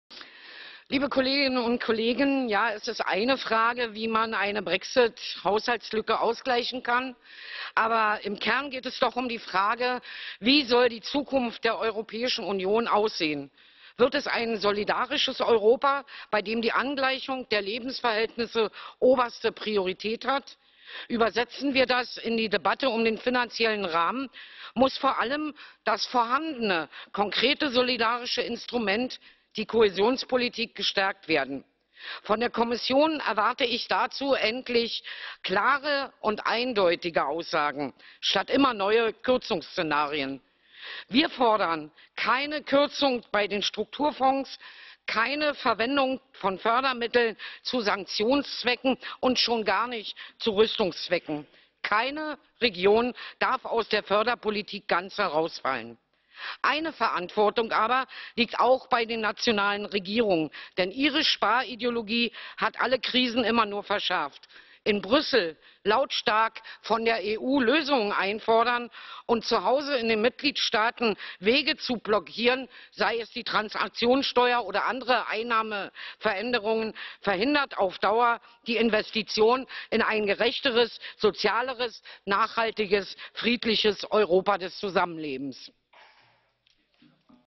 MdEP Martina Michels (DIE LINKE.) in der Debatte zum Mehrjährigen Finanzrahmen der EU ab 2020
Redemanuskript:
Kolleginnen und Kollegen,
Es ist eine Frage, wie man eine Brexit-Haushaltslücke ausgleichen kann.

ABER im Kern geht es um die Frage:
Was und wie soll die Europäische Union in Zukunft sein?
Für mich ist die Solidarität in Europa der wichtigste Teil der Antwort.
Solidarität übersetzt in EU-Politik bedeutet konkret auch:
Keine Kürzungen bei den Strukturfonds.
Keine Verwendung der Fördermittel zu Sanktionszwecken.
Keine Region darf aus Förderpolitik ganz herausfallen.
Das muss die rote Linie des Europaparlaments (in den MFR-Verhandlungen) sein.

Von der Kommission erwarte ich endlich ein klares Bekenntnis zur Kohäsionspolitik für alle, statt immer neue Kürzungsszenarien.

Die Hauptverantwortung aber liegt bei den Regierungen.
Ihre Sparideologie hat alle Krisen immer nur verschärft.
Es ist höchste Zeit, den Kurs zu ändern.
(In ein gerechteres, sozialeres, nachhaltiges, friedliches Europa Zusammenlebens zu investieren.)
Das sage ich klar auch den Regierungsparteien in Deutschland.
Ihr Koalitionsvertrag enthält die Verpflichtung auf eine starke Kohäsionspolitik für alle
und auf einen höheren Beitrag zum EU-Haushalt.
Daran werden wir Sie erinnern und messen.